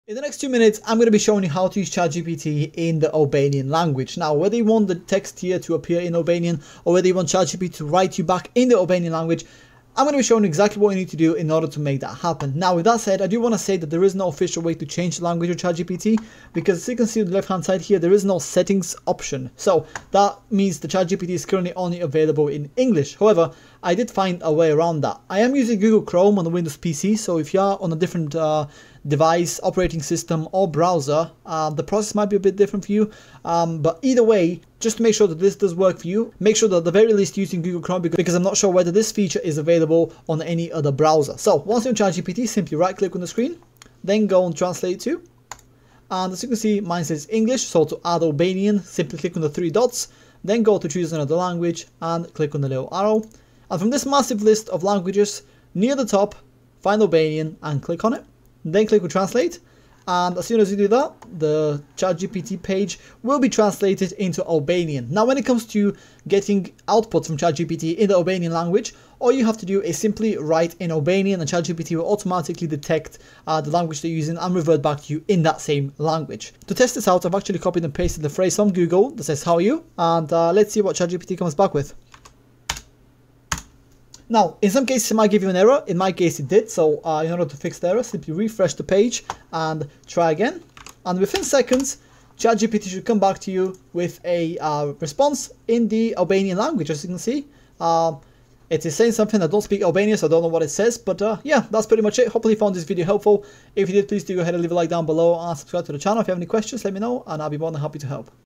How to Use Chat GPT in Albanian in 2024
In the next 2 minutes, I'll be showing you everything you need to do to start using Chat GPT in the Albanian language.